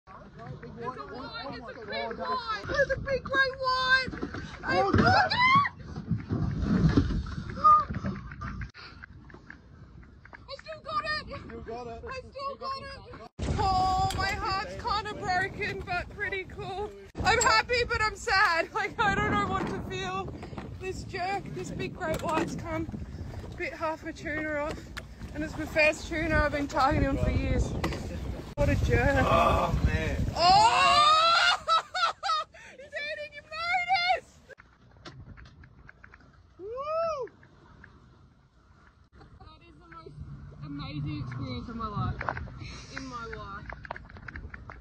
Great white shark takes a chunk out of woman's tuna
A woman was delighted at the prospect of landing a big tuna while fishing, but a hungry greatwhite shark made things a bit more complicated and bloody.